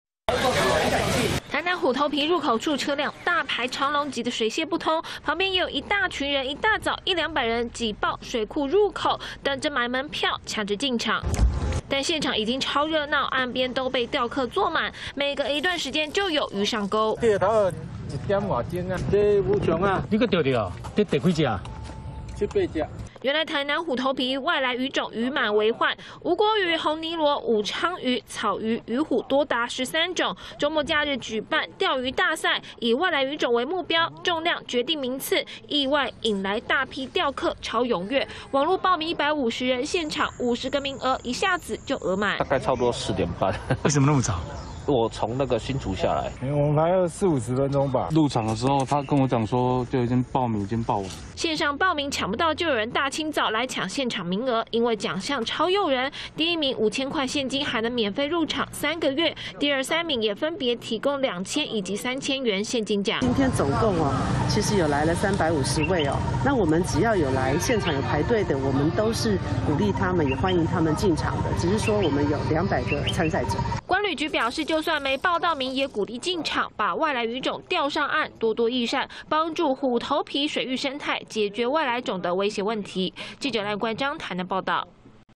清晨就來! 搶拿5千獎金 釣客擠爆虎頭埤入口│中視新聞 20220327
台南虎頭埤水庫，長期受到外來魚種入侵，就怕原生魚種滅絕，台南市觀旅局舉辦外來魚種釣魚大賽，依重量決定名次，第一名有5千元現金，加上釣魚月票三個月，意外引來大批釣客進場，一早6點多 上百人擠爆入口處，還有從新竹來的釣客，清晨4點半就來卡位!